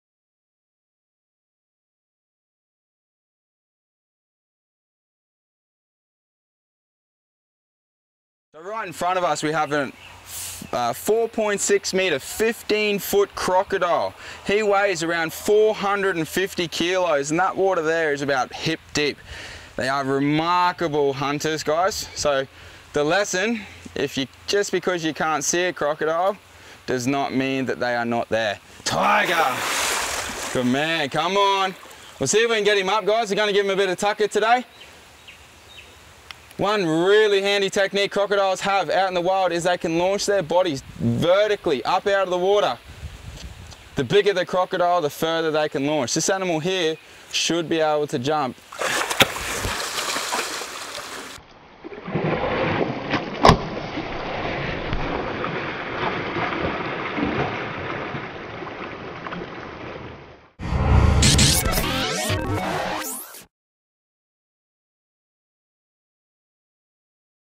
15 foot Hungry Crocodile vs Man with Food, Australia Queensland
Here's a video from our vault of a 15 foot hungry saltwater crocodile being hand fed a chicken in North Queensland, Australia.

"PLEASE DO NOT ATTEMPT THIS"

Special thanks to Johnson River Crocodile & Animal Park.